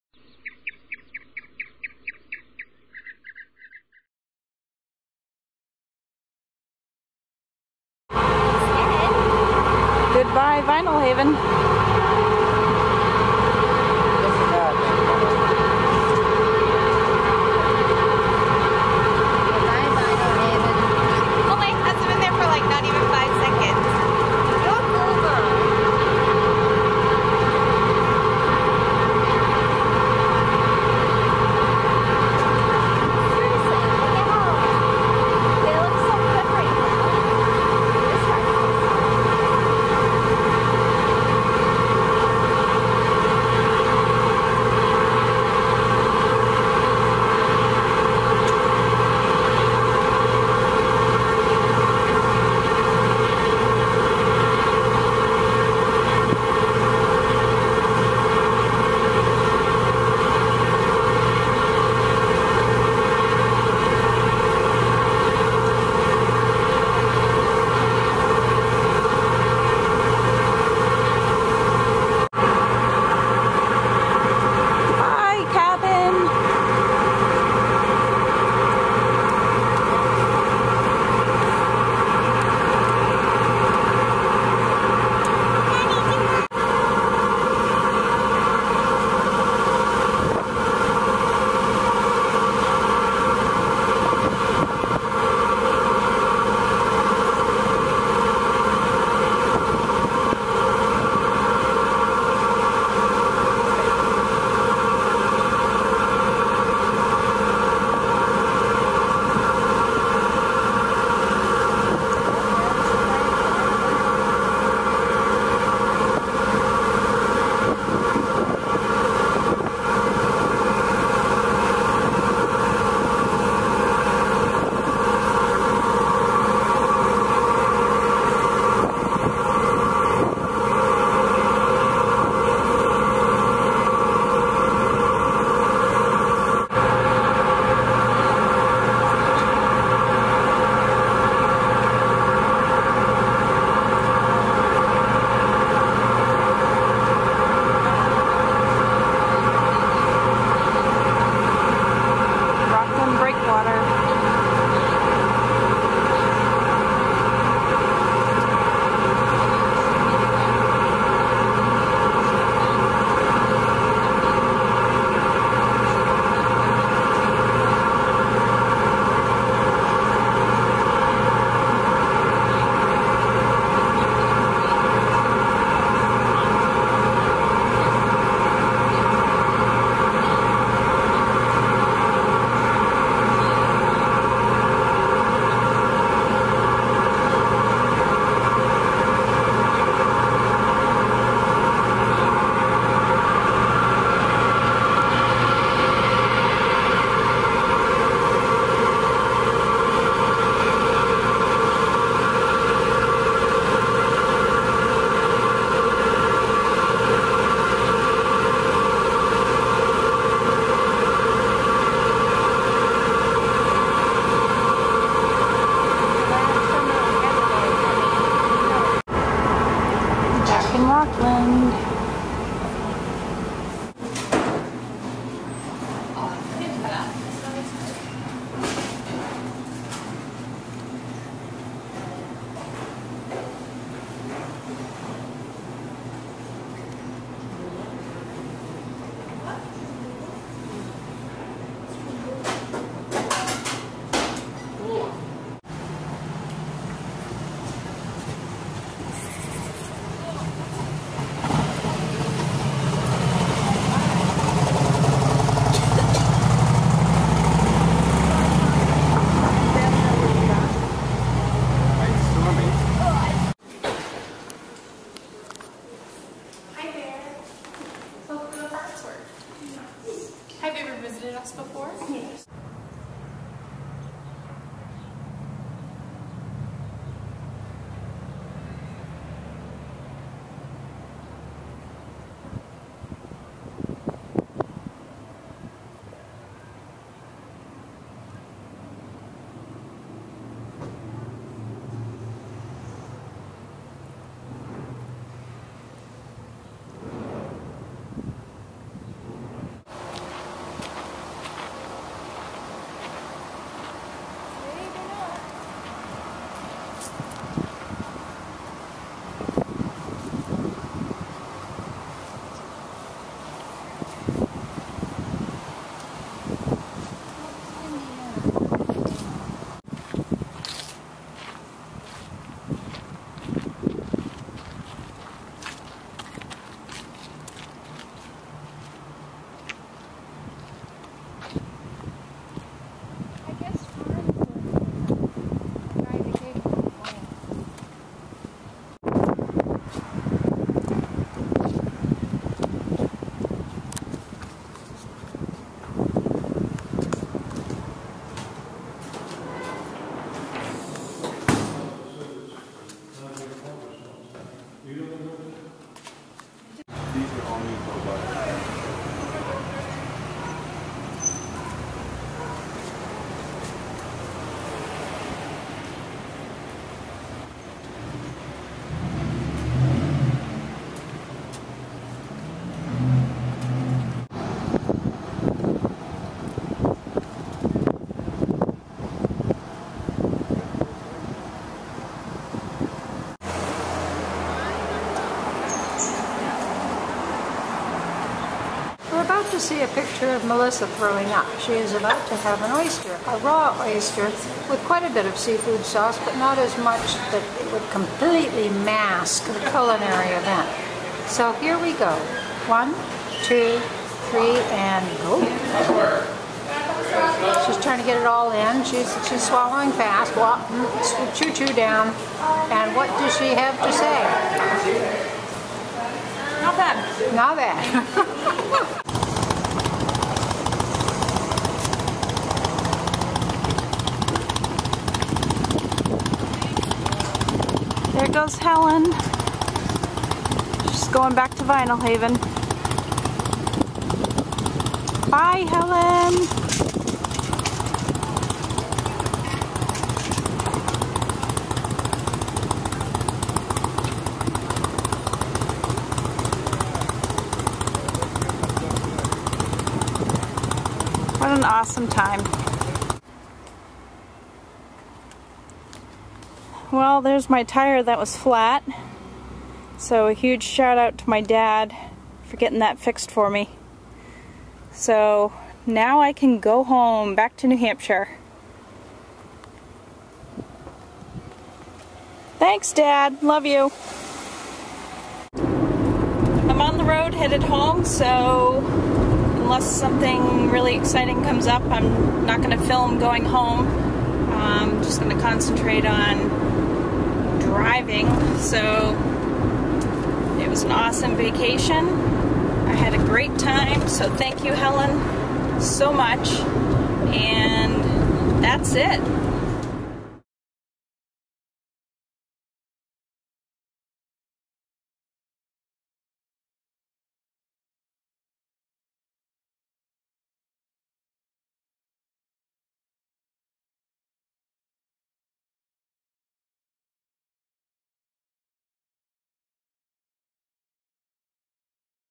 Vinalhaven [Day 5]: Returning to Rockland
Welcome back to my travel vlog! In this episode, I am heading back to Rockland, Maine for day 5 and the final episode of my adventure. Join me as I start the day with a ferry ride, a delicious cup of coffee and mouthwatering muffin. Helen and I make our way past the iconic Strand Theater. Next, we explore the renowned Farnsworth Art Museum, marveling at its incredible collection of Wyeth art. But the highlights don't end there! Watch as I muster up the courage to try my first (and last) raw oyster on a half shell, a memorable dining experience to say the least. Oh, and let's not forget about Dad coming to the rescue! After encountering a flat tire, he heroically drives six hours to fix it.